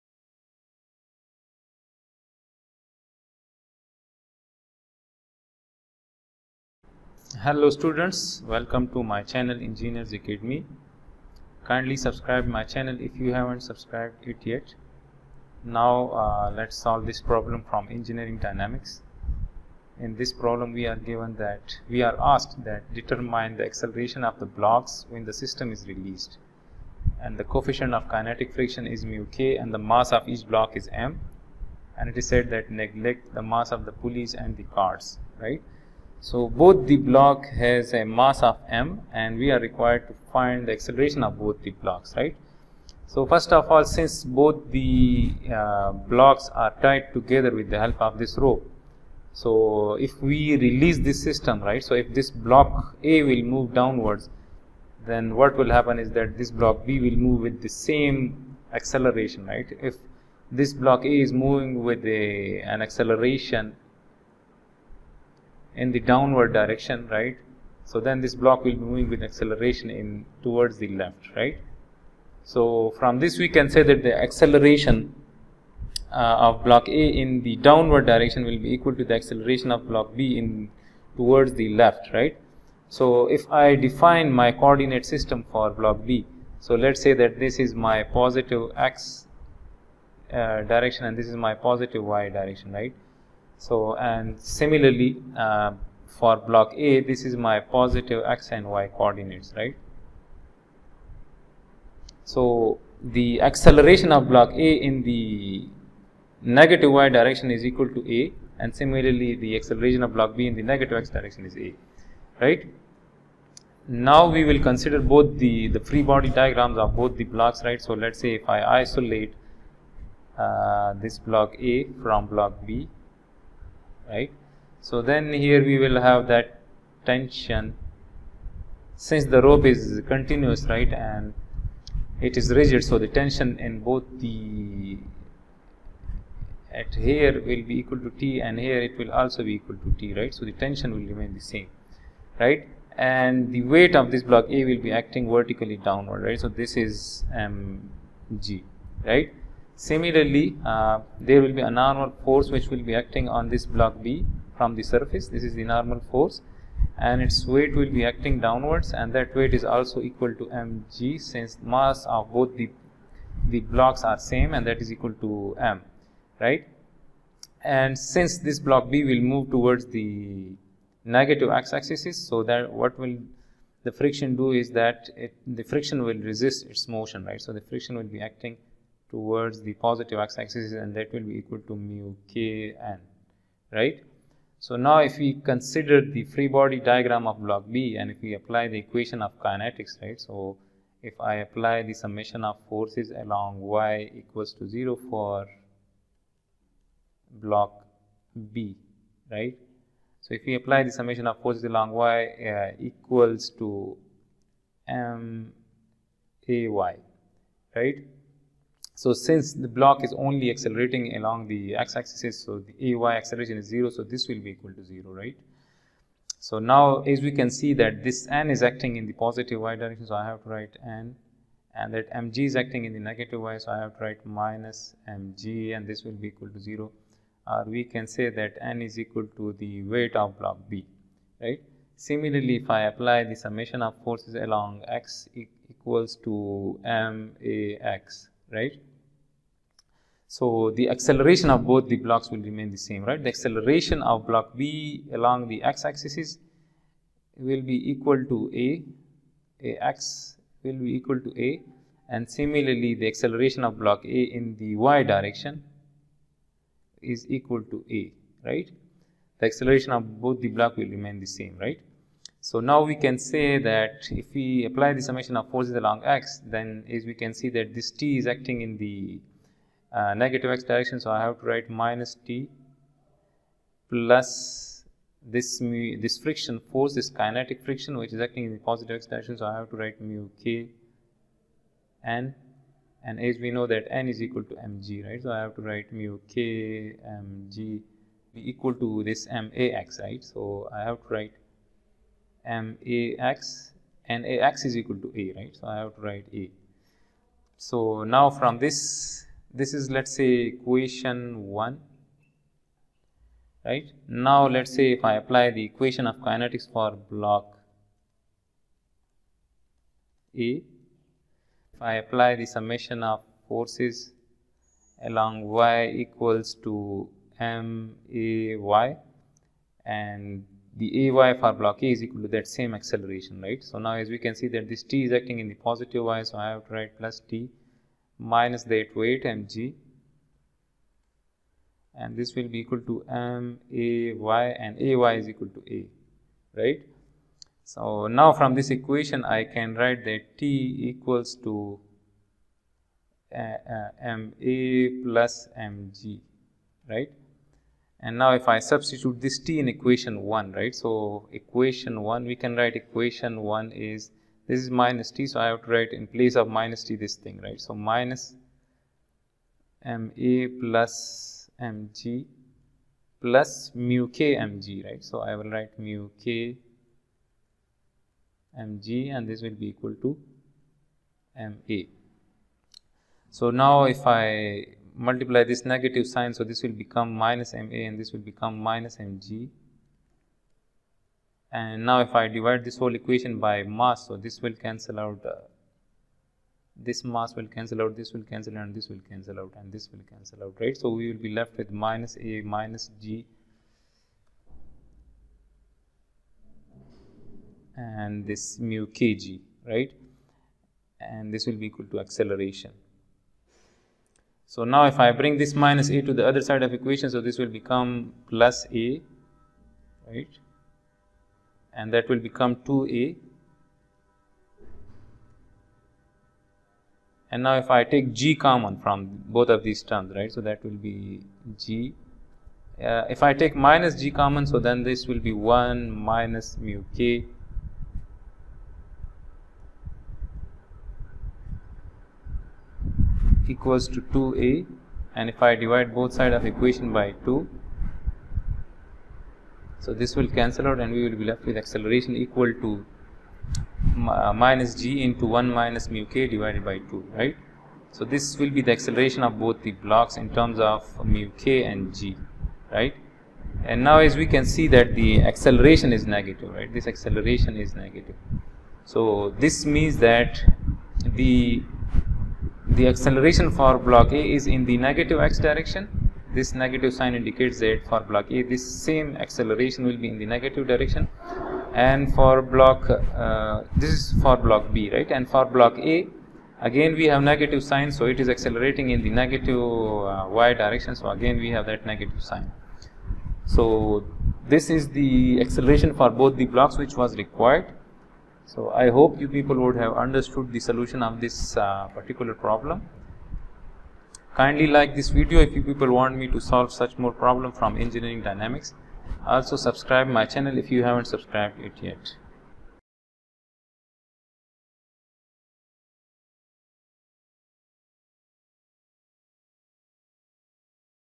13-17 | Kinetics of a Particle | Chapter 13: Hibbeler Dynamics 14th ed | Engineers Academy
Chapter 13: Kinetics of a Particle : Force and Acceleration
Hibbeler Dynamics  14th ed

13–17. Determine the acceleration of the blocks when the system is released. The coefficient of kinetic friction is uk, and the mass of each block is m. Neglect the mass of the pulleys and cord.